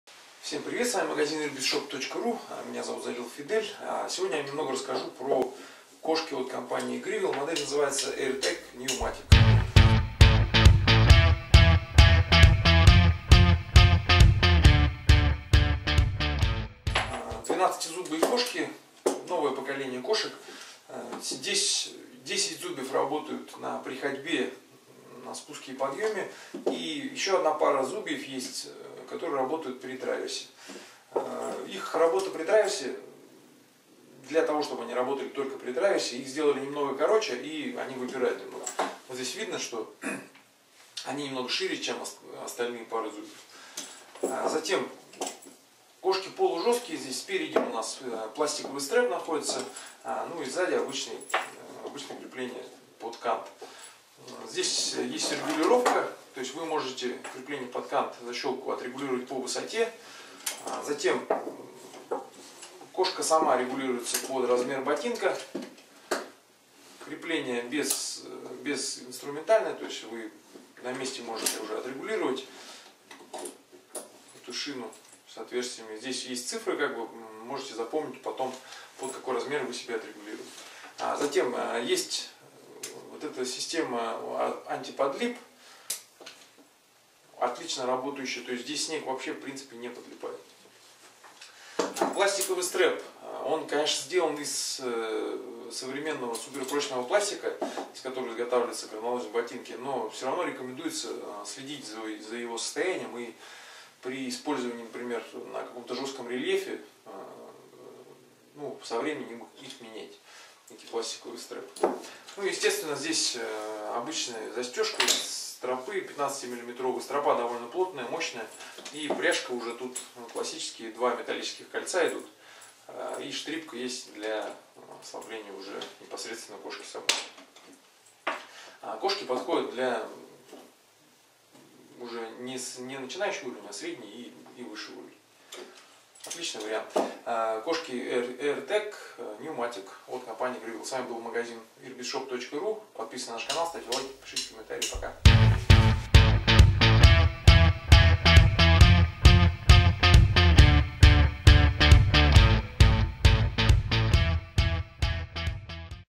Кошки Grivel Air Tech New Matic
Обзор альпинистских кошек Grivel Air Tech New Matic от интернет-магазина туристического снаряжения "Ирбис"